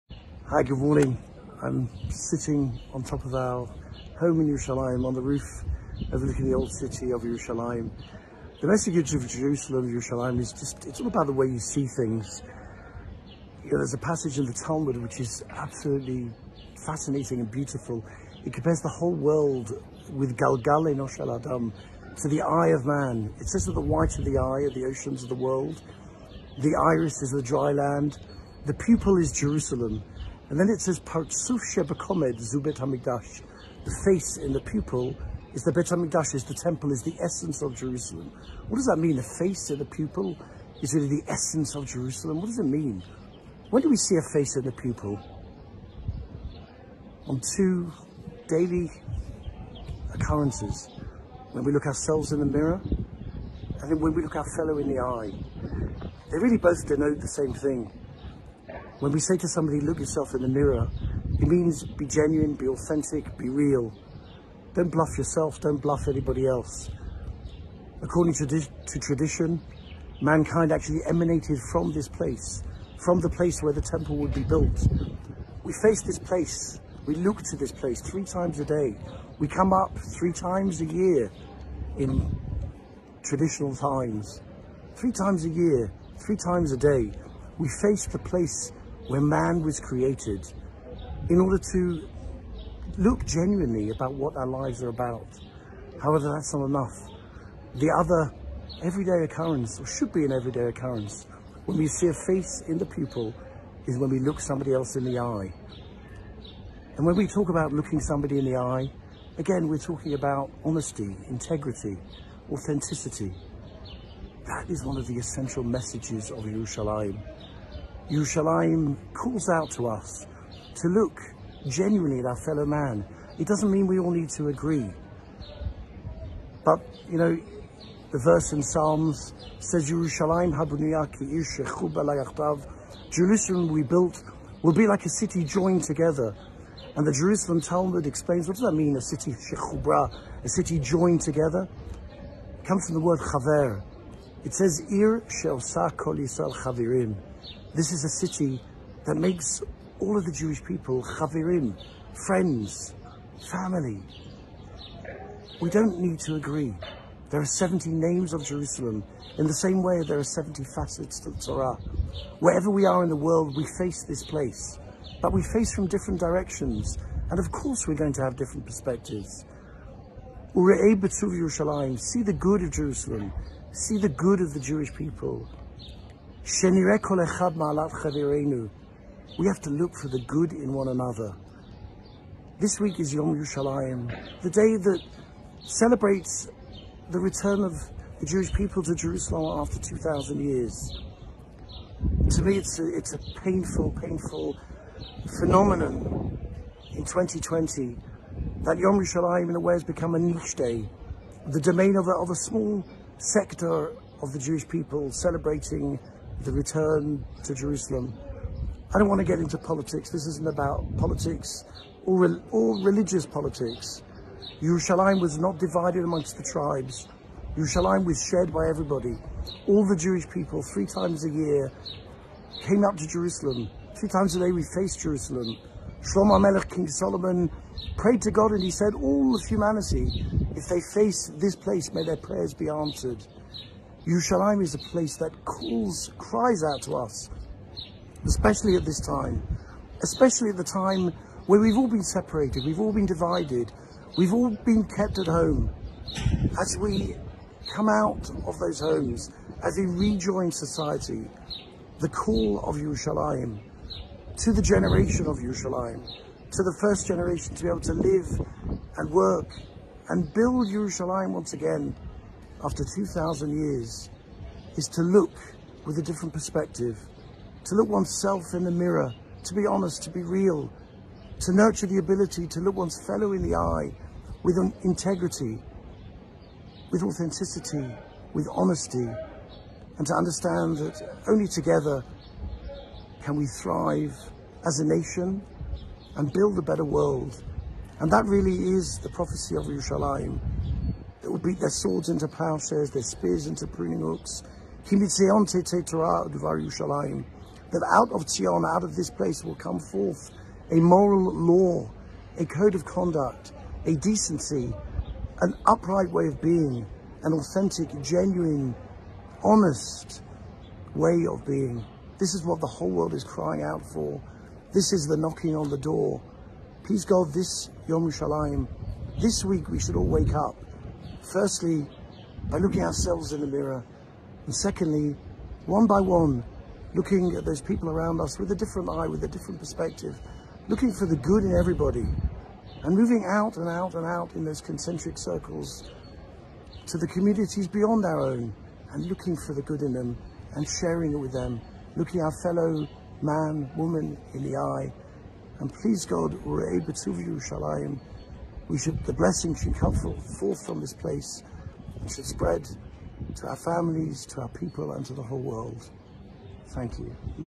Jerusalem and Togetherness
What can Jerusalem teach us about togetherness?

As we approach Yom Yerushalayim, JRoots Journeys co-founder Rabbi Naftali Schiff shares a connection between the day, the city, and our New Normal from atop the Old City. Perhaps, when we can experience this magnificent view for ourselves again, the call to look4thegood will make the experience even more meaningful.